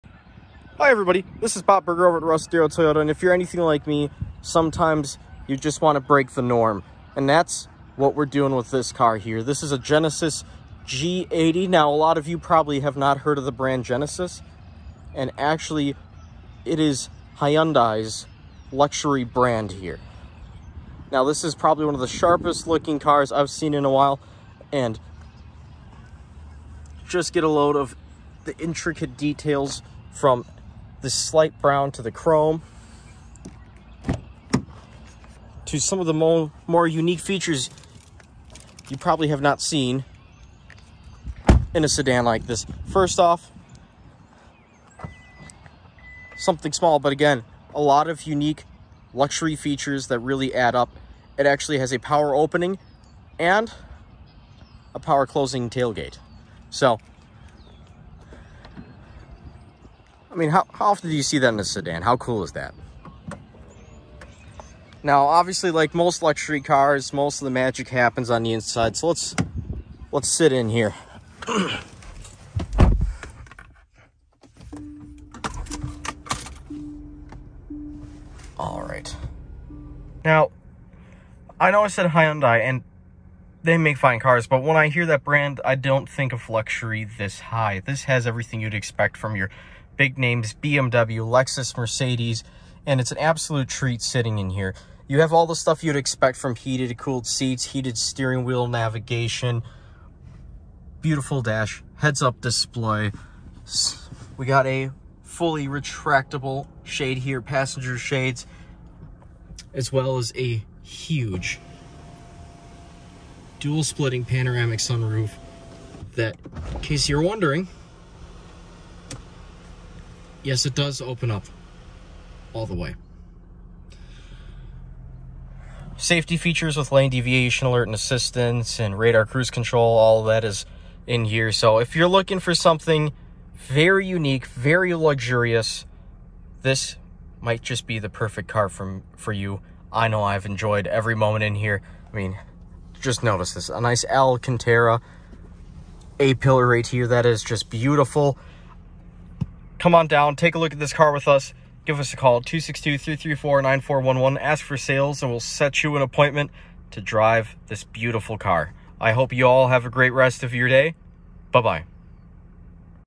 What Car Would I Buy with Russ Darrow Toyota
What vehicle would our darrowdifferencemakers want to buy? Come and see for yourself! If you have any questions about the featured models or any other inventory, feel free to stop in or give us a call today